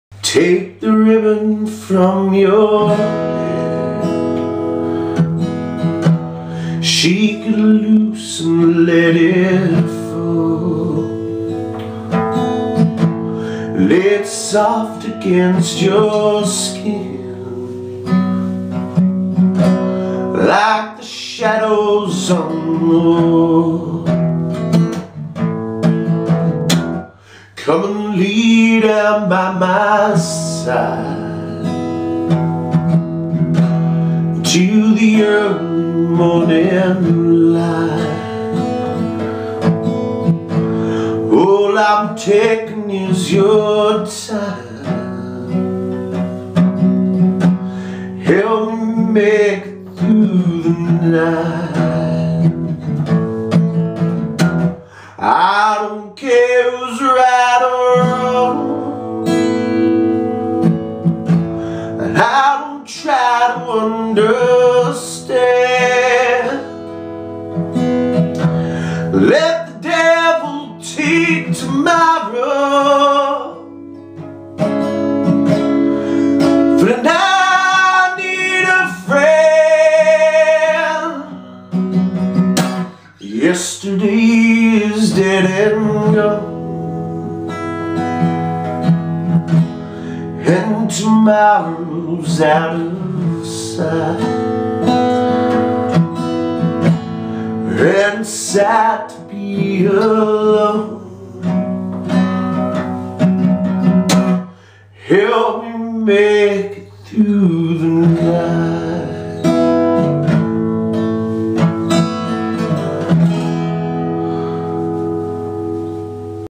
Both types of music here
A little country and western ballad this afternoon. Hope you're all well. Talk to you tomorrow.